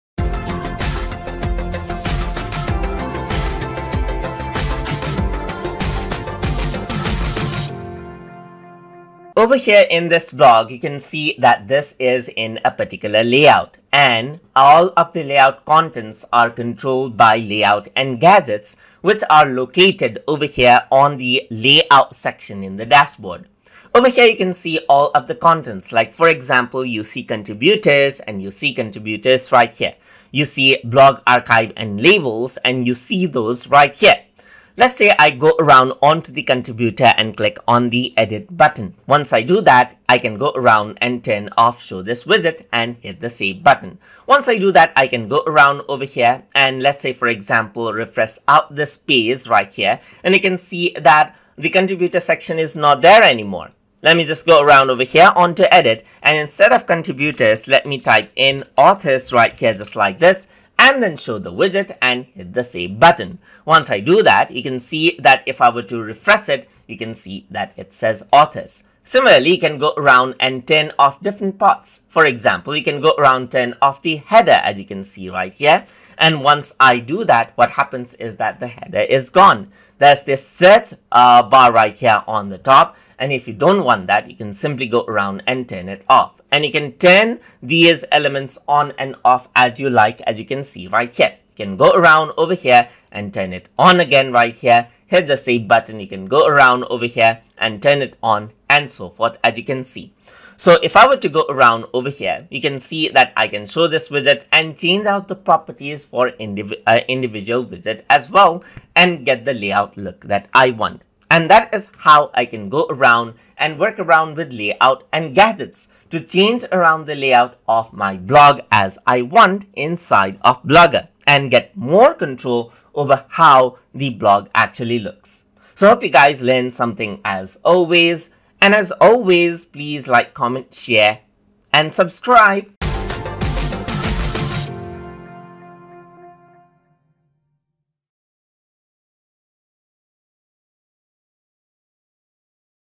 Blogger - Lesson 54 - Layout and Gadgets
In this tutorial, we will be discussing about Layout and Gadgets in Blogger

Learn the basics of using Blogger including how to work start start edit and manage blogs.

This lesson has been made using Blogger in Google Chrome Browser but is applicate for other supported browsers as well.